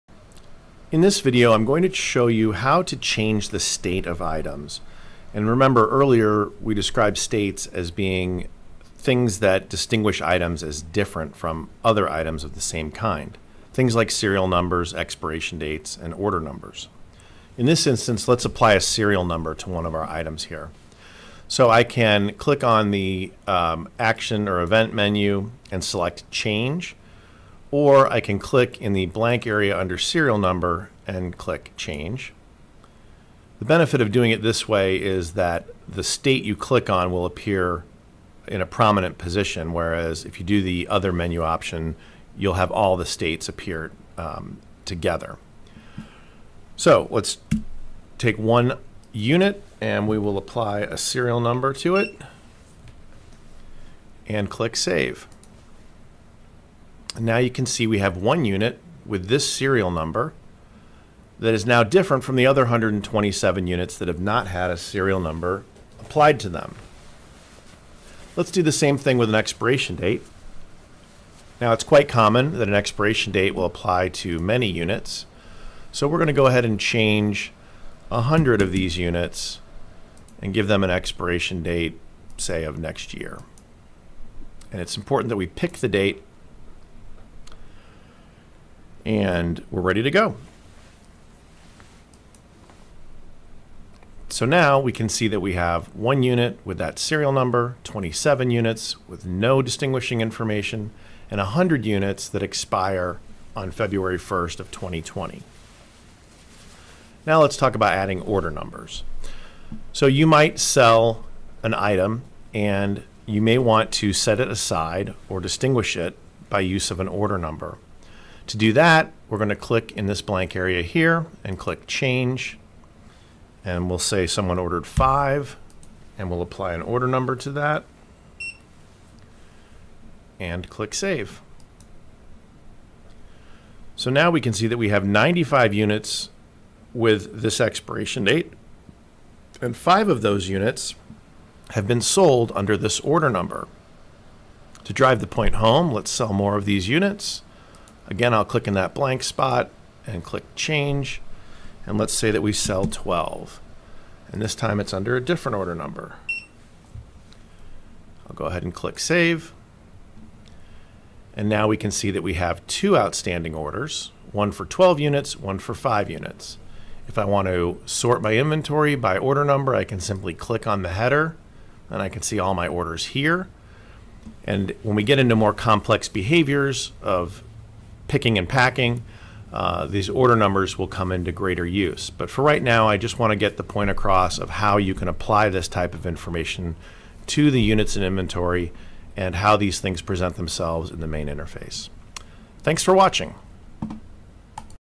1010 - Changing the Serial Number, Expiration Date, or Order Number of Items
This video will show you how to apply serial numbers, expiration dates, or order numbers to units of items in inventory.